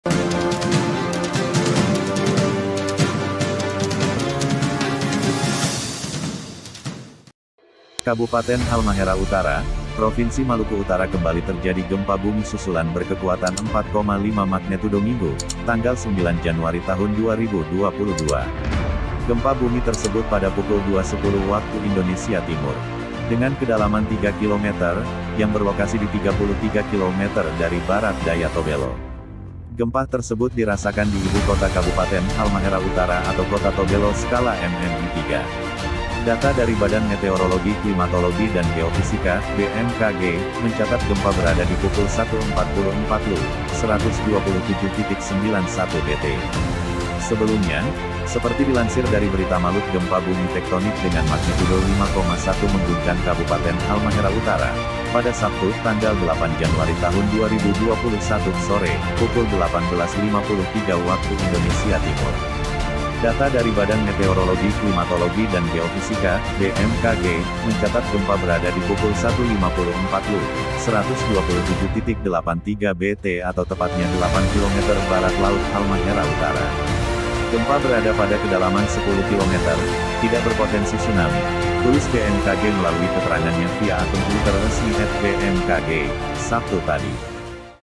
Halmahera Utara Kembali Terjadi Gempah Bumi Susulan Berkekuatan 4,5 Magnetudo II Jejakmalut
Kabupaten Halmahera Utara, Provinsi Maluku Utara kembali terjadi gempa Bumi susulan  berkekuatan 4,5 magnetudo Minggu, (9/1/2022).

Gempa Bumi tersebut pada pukul 02.10 WIT. Dengan kedalaman 3 kilo meter, yang berlokasi di 33 Kilo meter dari Barat Daya Tobelo.

Gempah tersebut dirasakan di Ibu Kota Kabupaten Halmahera Utara atau Kota Tobelo  skala MMI III.

Data dari Badan Meteorologi Klimatologi dan Geofisika (BMKG) mencatat gempa berada di 1.44 LU,127.91 BT.

# Gempah Maluku Utara#.

TERIMAKASIH : Kru Jejakmalut